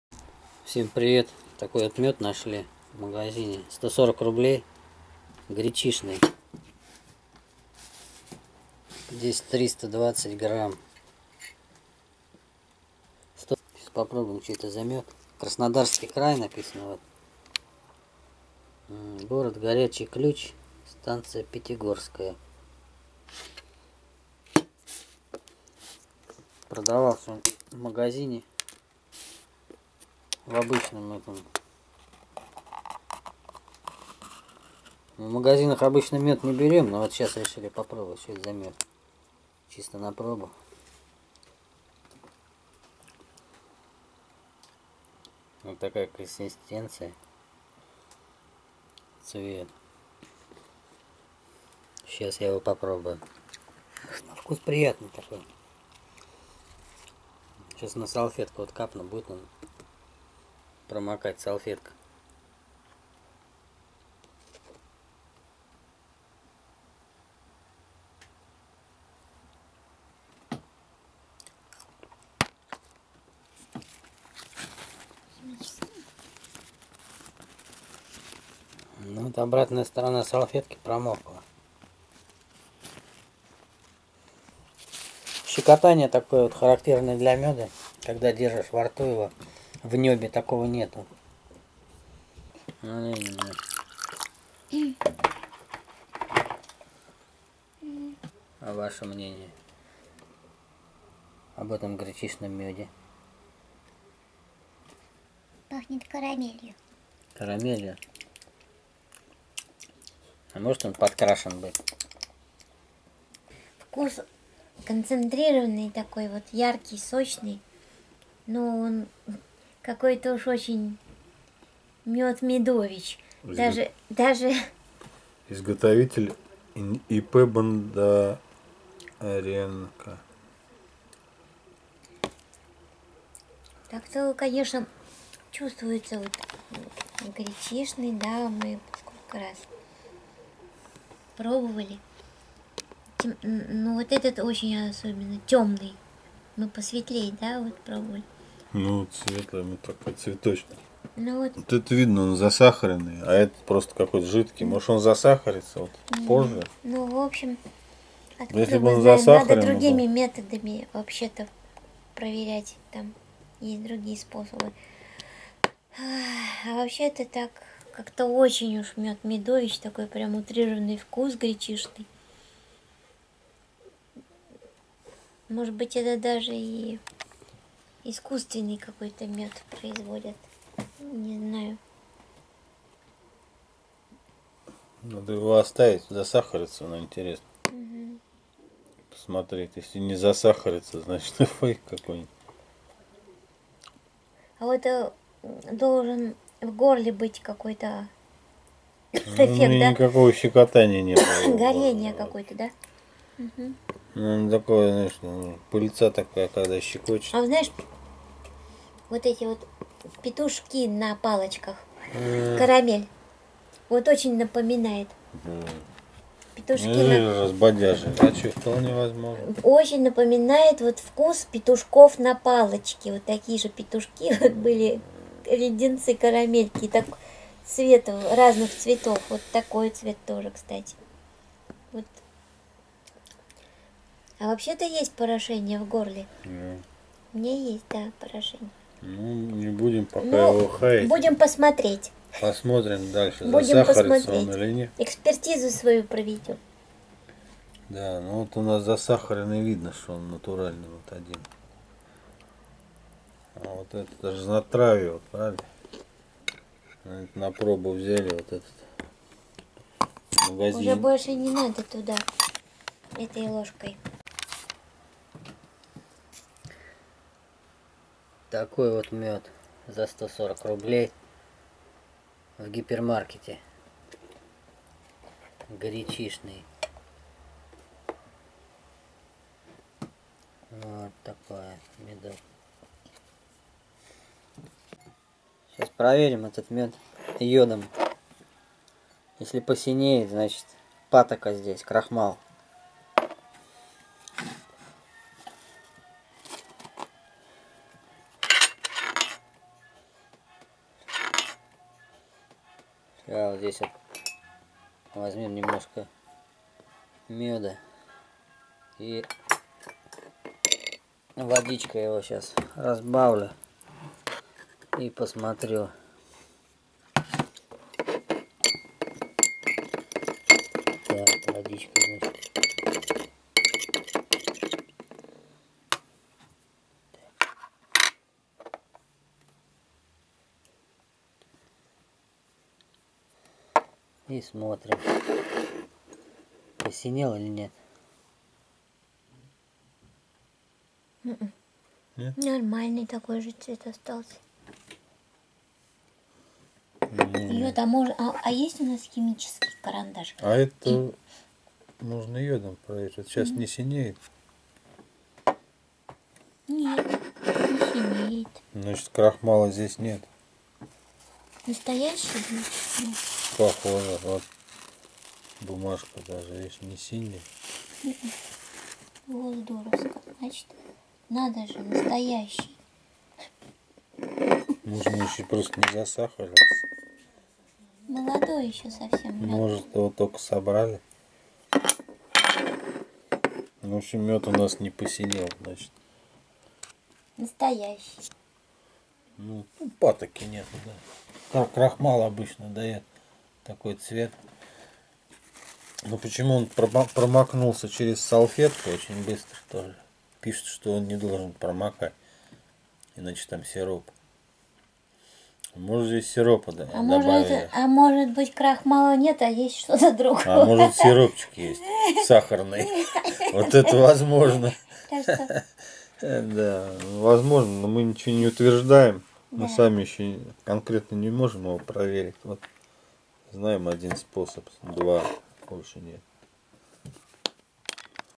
Горчичный мёд? Пробуем натуральный горчичный мёд, купленный в магазине. Проверка мёда йодом.
Купили горчичный мёд в магазине. На банке написано натуральный.